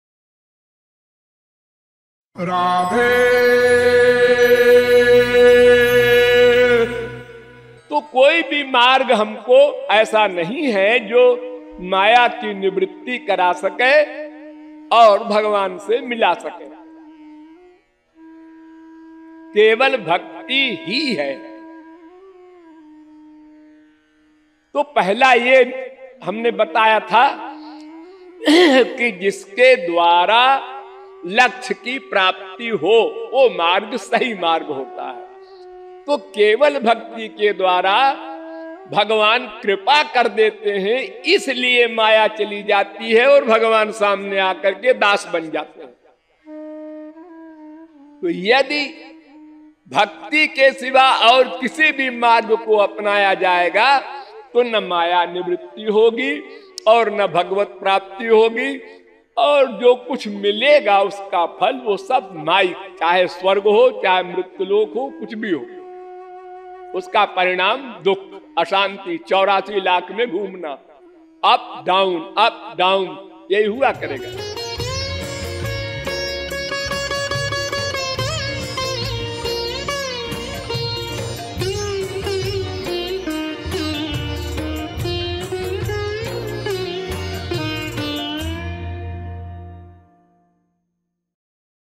भगवान से मिलने का एकमात्र मार्ग : Jagadguru Shri Kripalu Ji Maharaj | Short Pravachan
जगद्गुरु श्री कृपालु जी महाराज के श्री मुख से : भगवान से मिलने का एकमात्र मार्ग
Jagadguru Shri Kripalu Ji Maharaj | Short Pravachan